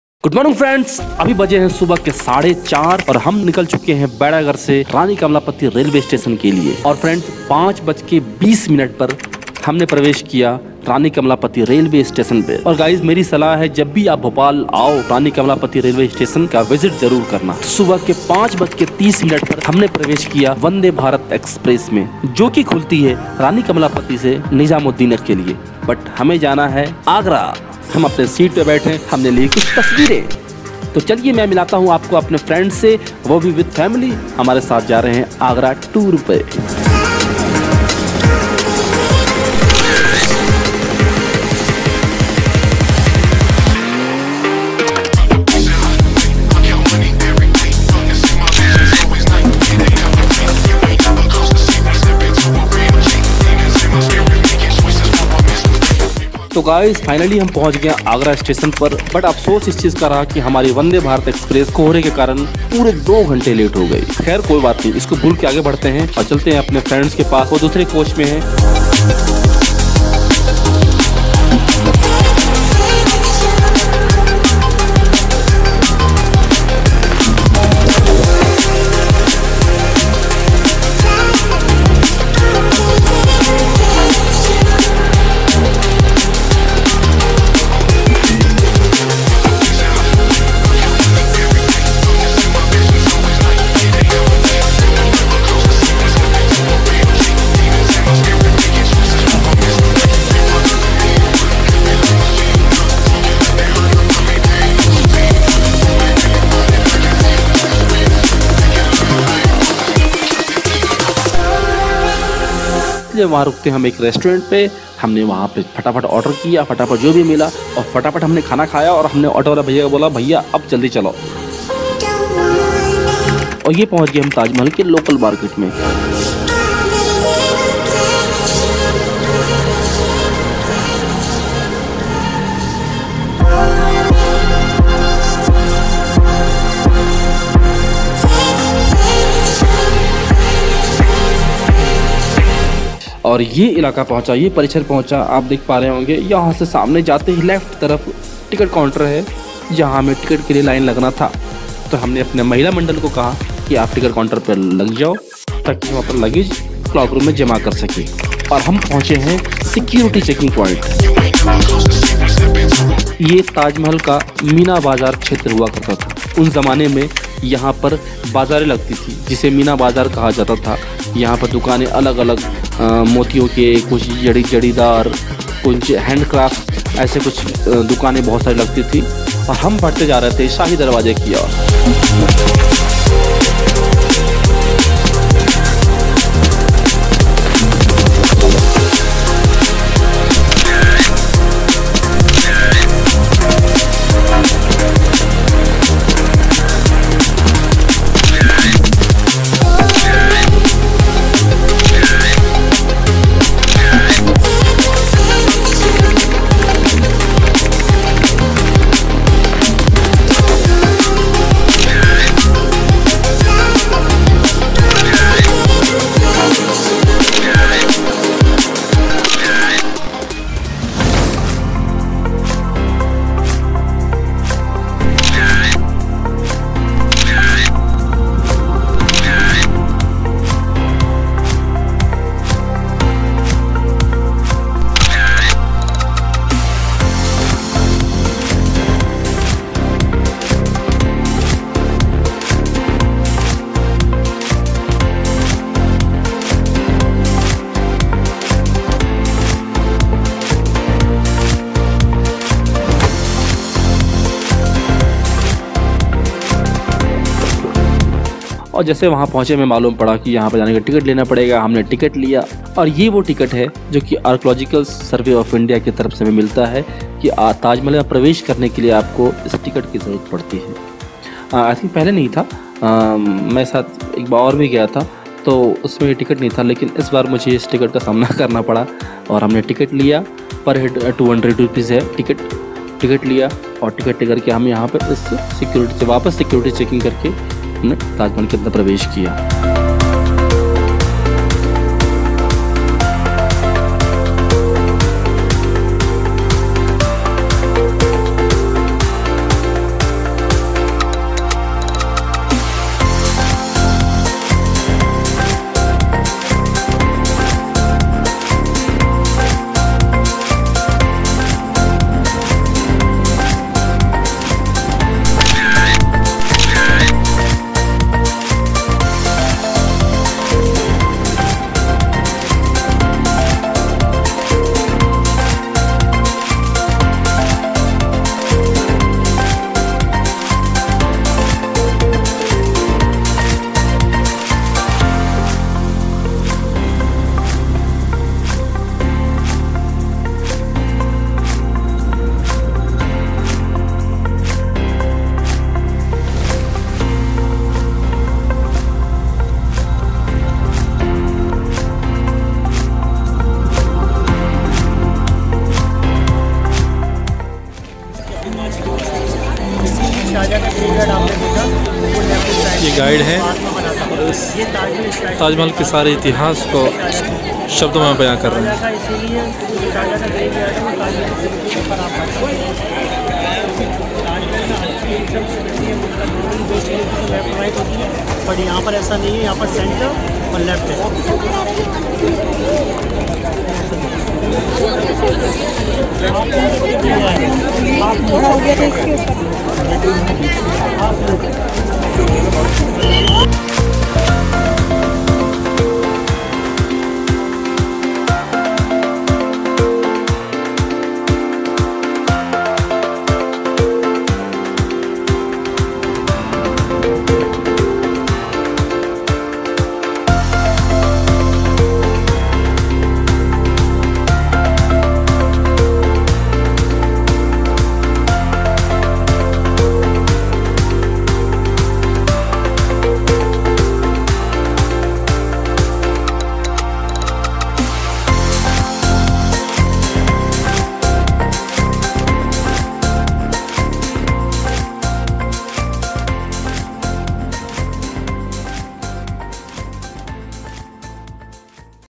TAJMAHAL TOUR VLOG 🇮🇳 Rajmani Production
We visited Great Tajmahal with my family and friends Tajmahal is really fantastic place to visit again and again
such The great Tajmahal made by Mugal emperor Shahjahan for our lovely wife Mumtaz Begam.
Architecture of Tajmahal was Great Architecture Ustad Ahmed Lahori such Great job 👍
I really enjoyed that place to visit with my family and friends I found lot of Amazing art on Tajmahal campus Like Shahi door , Great 4 Piller everything was Great wah Taj wah.
I shoot all these videos in my Smartphone happyli and Edit after the tour